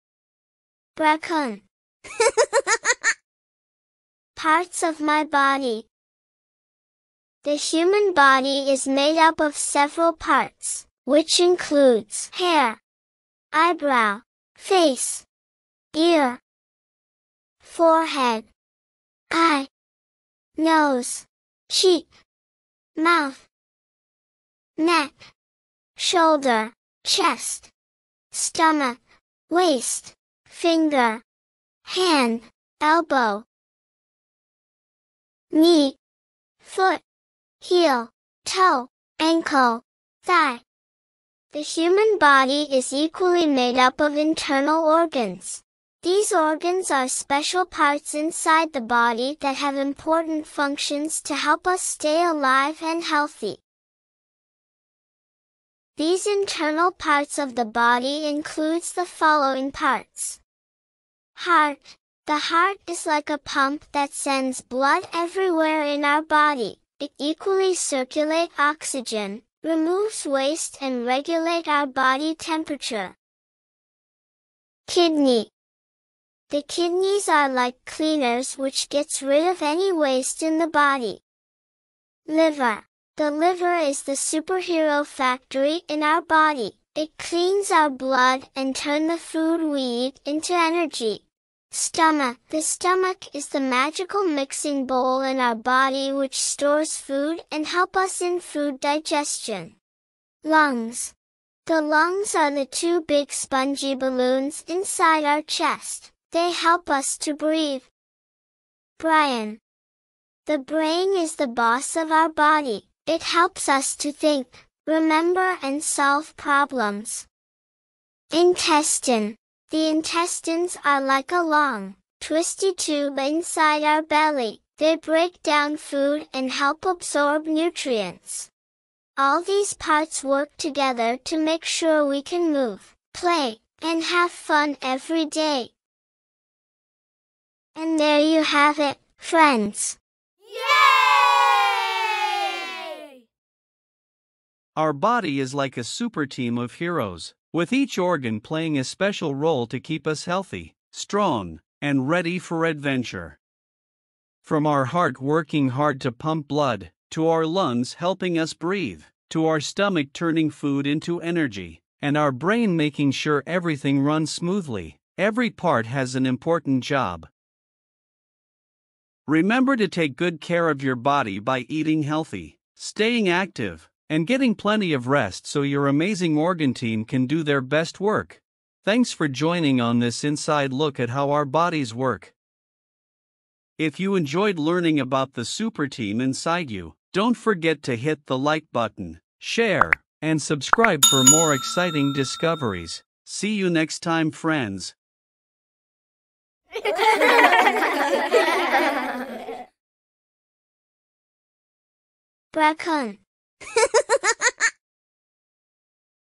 The Essential Guide to Understanding the Human Body Parts
The Essential Guide to Understanding the Human Body Parts is a captivating exploration designed to illuminate the intricate details and fascinating functions of our bodies. This guide takes you on a comprehensive journey through each vital component that makes up the human body. Discover the roles and importance of various organs, muscles, and systems that work tirelessly to keep us alive and thriving. With engaging visuals and expert explanations, this video is perfect for anyone looking to Understanding the Human Body Parts, appreciate the marvels of our physical structure, and understand how to care for and maintain their health.